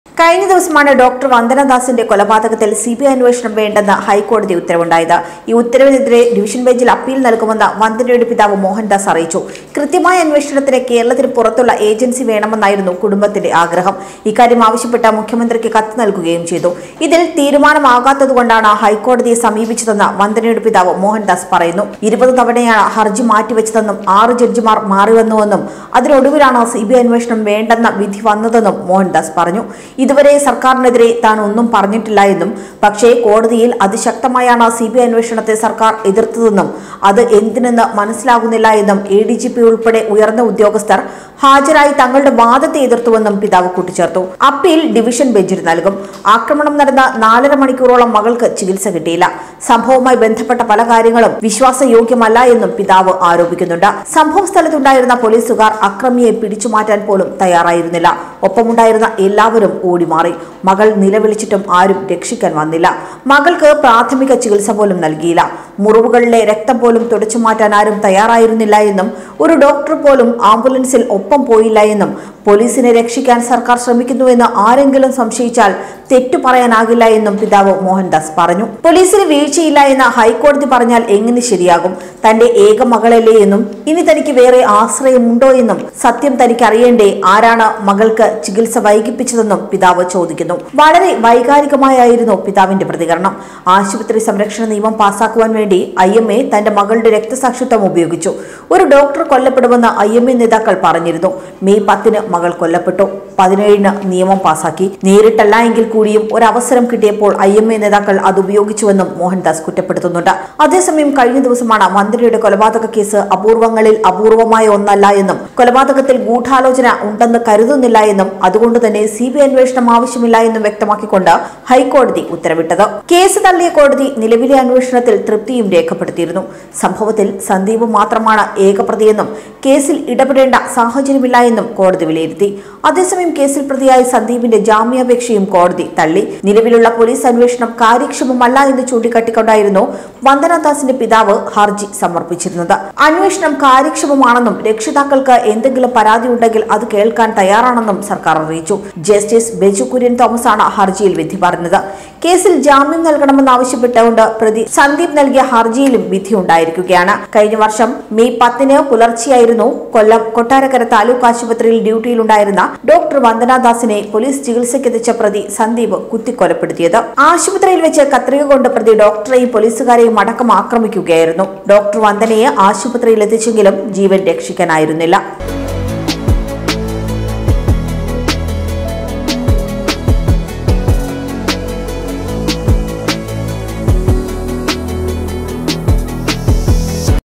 ഞങ്ങൾക്ക് വേറെ ആശ്രയമില്ല; സത്യം അറിയണമെന്ന് ഡോ വന്ദനയുടെ അച്ഛൻ
ആക്രമണം നടന്ന് നാലര മണിക്കൂറോളം മകൾക്ക് ചികിൽസ കിട്ടിയില്ല. സംഭവസ്ഥലത്ത് ഉണ്ടായിരുന്ന പൊലീസുകാർ അക്രമിയെ പിടിച്ചു മാറ്റാൻ പോലും തയാറായില്ല. സംഭവവുമായി ബന്ധപ്പെട്ട പല കാര്യങ്ങളും വിശ്വാസ യോഗ്യമല്ലെന്ന് പിതാവ് ആരോപിക്കുന്നു